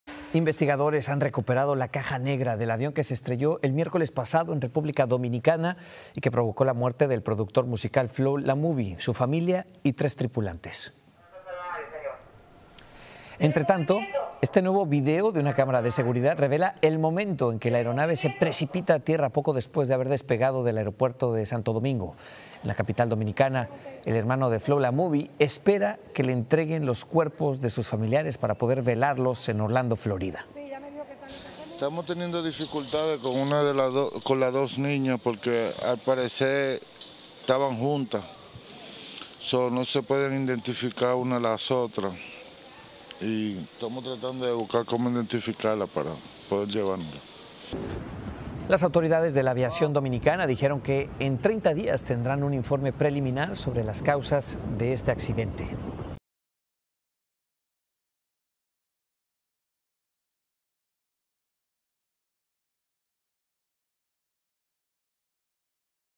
Nuevo video del avión de Flow La Movie cuando se estrella | Noticias Telemundo
Video oficial de Noticias Telemundo. Una cámara del aeropuerto muestra otro ángulo del impacto que terminó con la vida del productor musical, su familia y los pilotos del avión. La caja negra ya fue hallada, lo que facilitará a los investigadores determinar las causas del accidente.